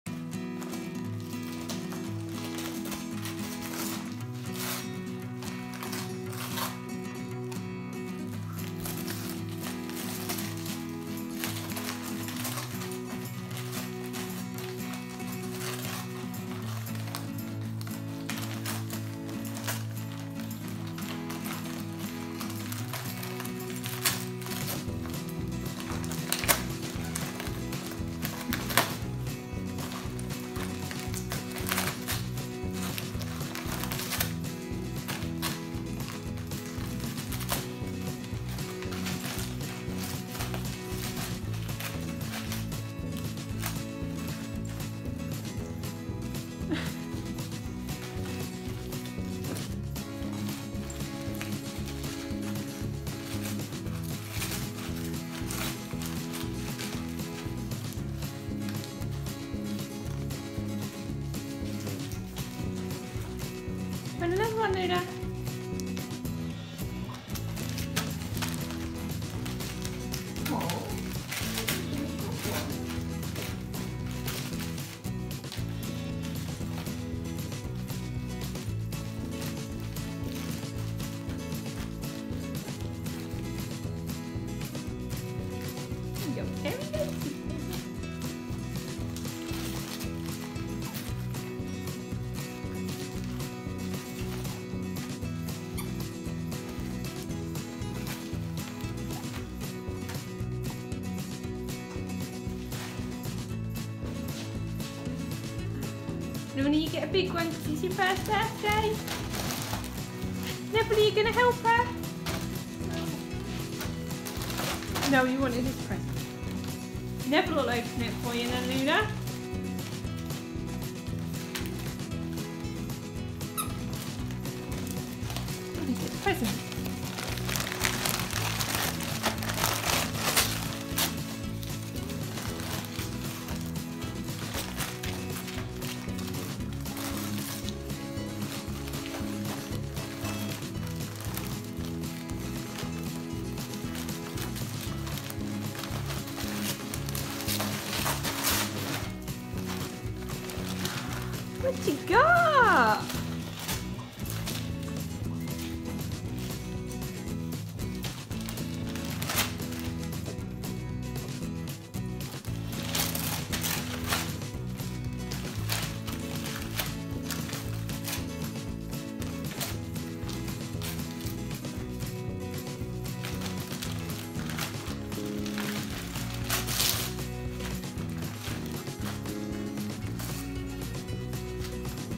Dogs opening their birthday presents
Neville the rescue dog and Luna the Springer Spaniel opening their barkday presents!